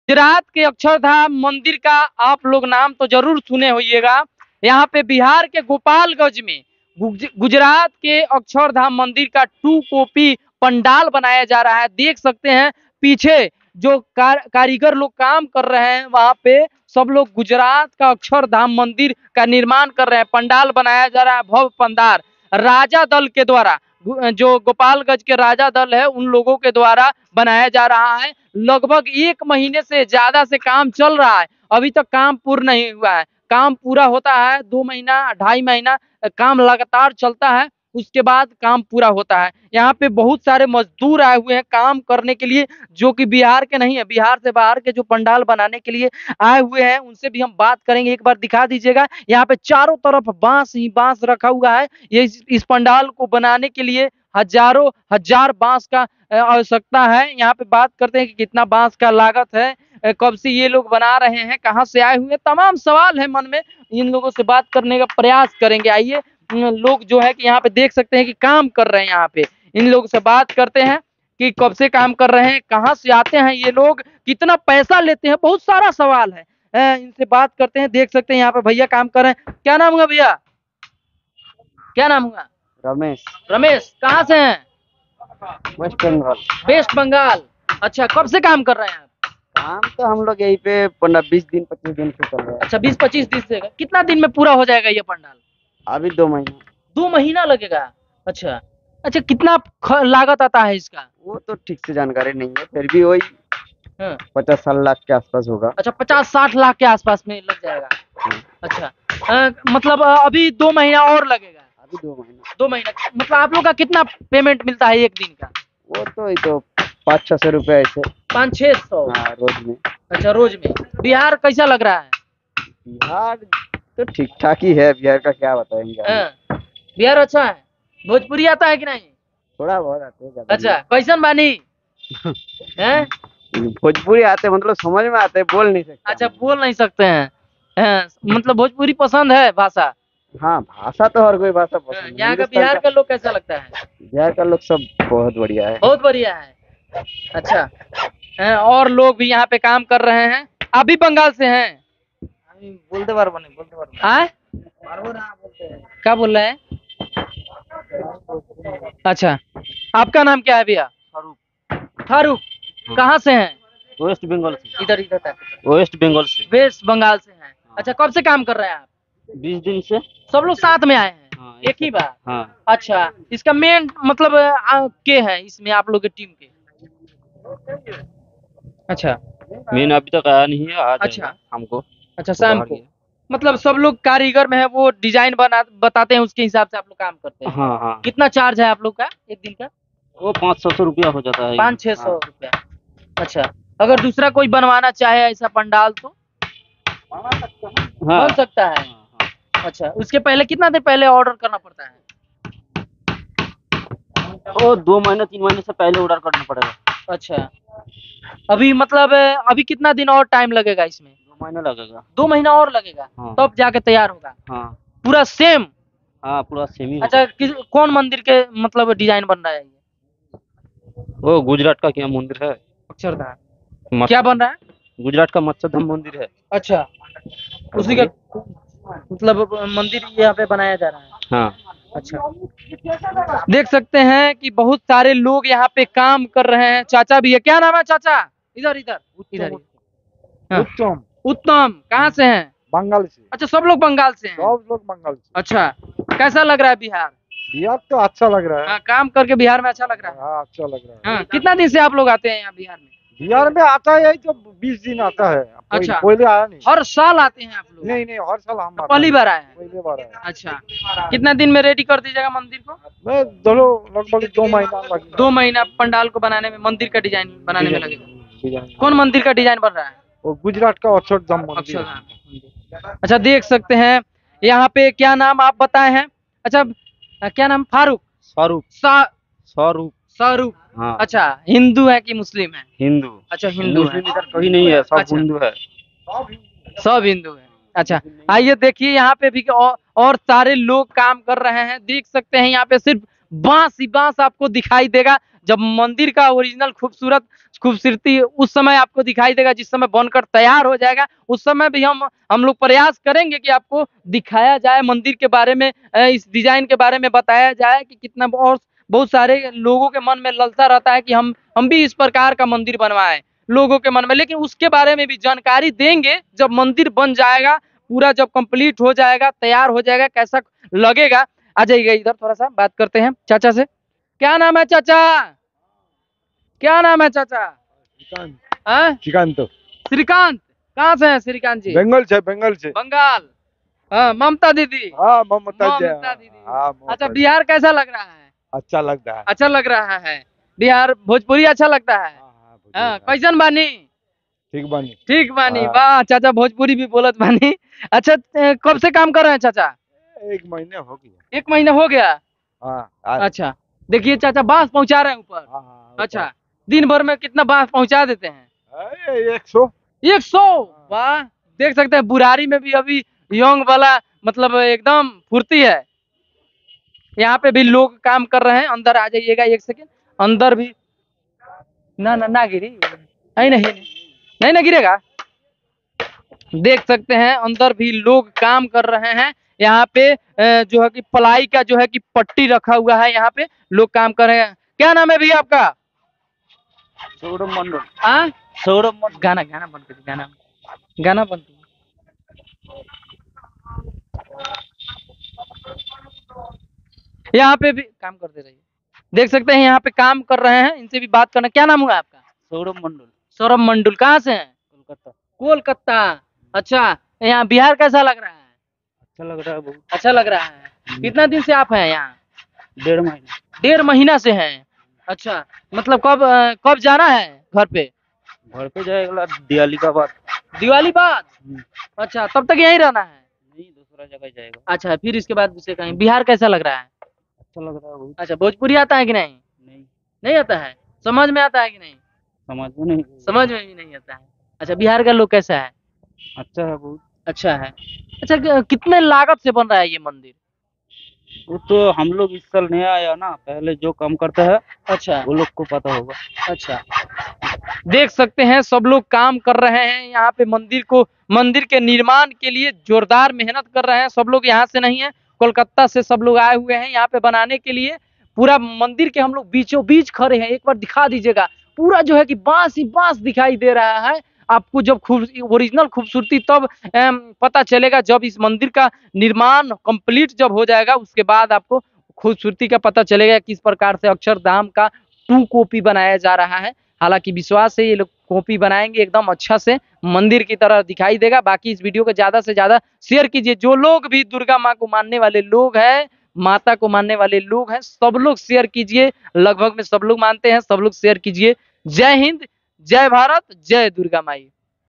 70 लाख की लागत से बन रहा, दुनिया का सबसे बड़ा दुर्गा पंडाल बिहार में
70 लाख की लागत से बन रहा, दुनिया का सबसे बड़ा दुर्गा पंडाल बिहार में